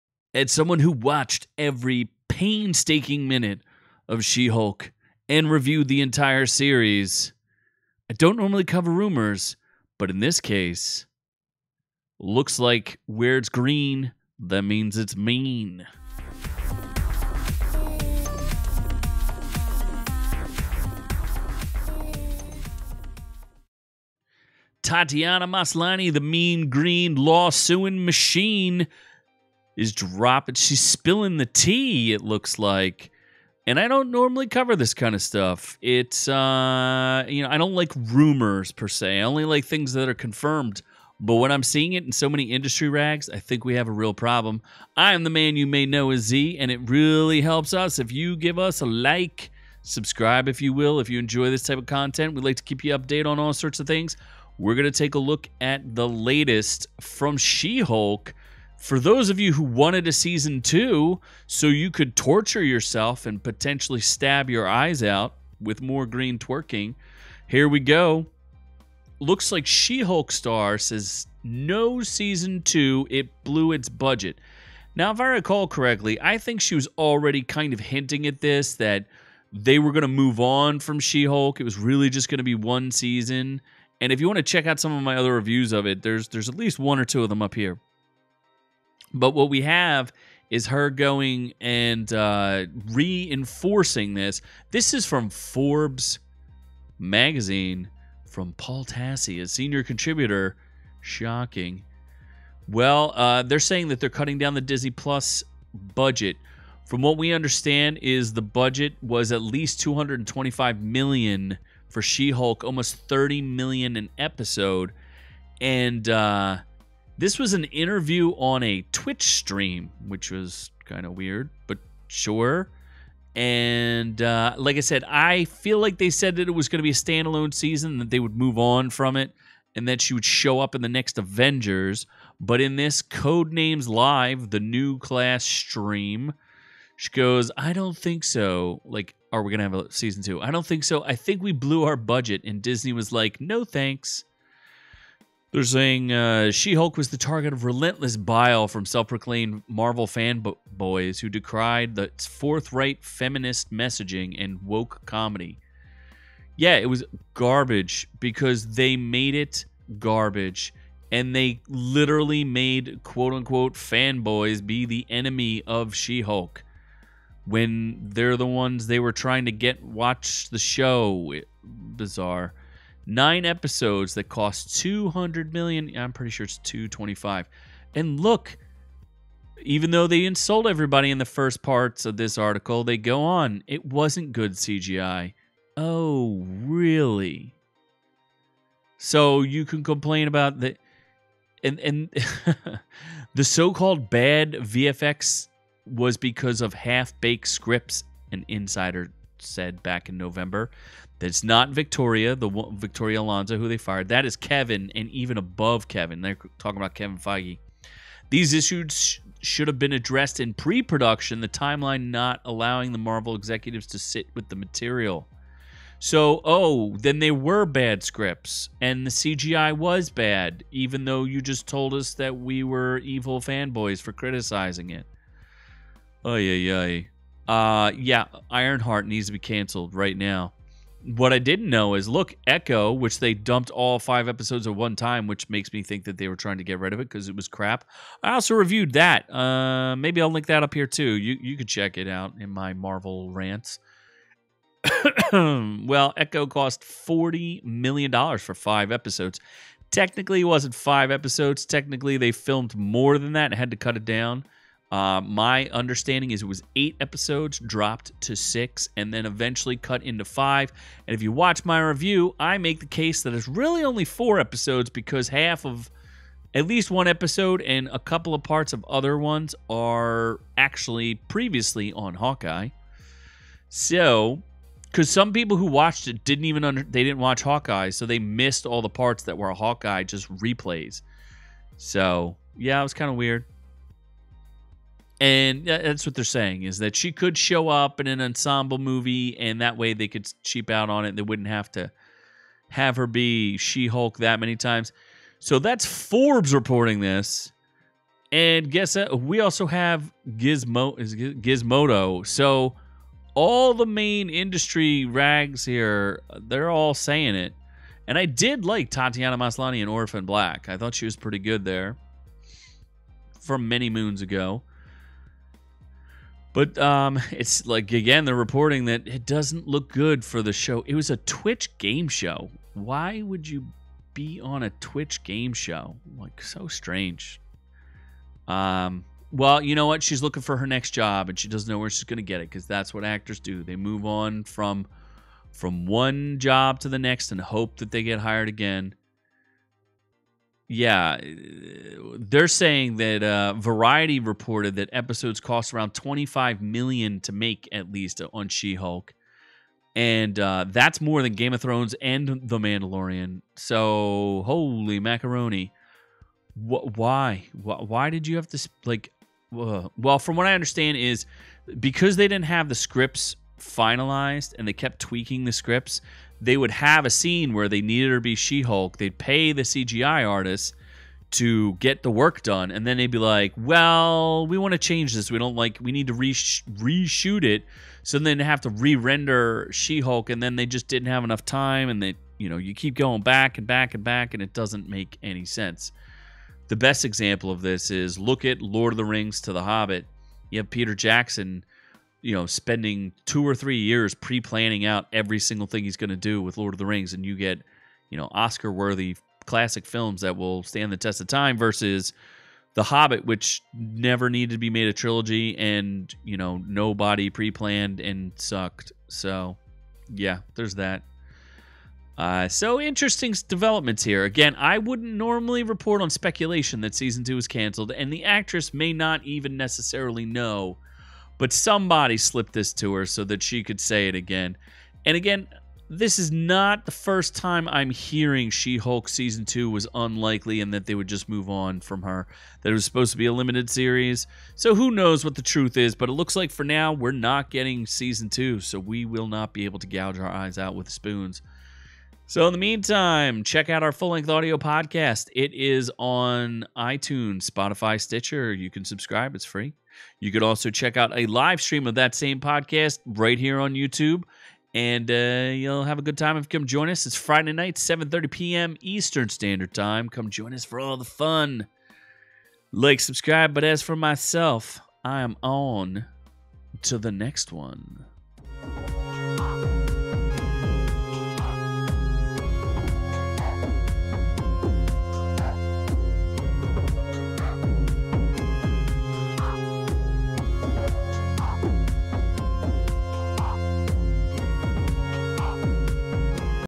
She Hulk Canceled? | Season 2 Unlikely
Z looks at She Hulk being canceled shehulkattorneyatlaw on shehulkdisney
Did you enjoy it?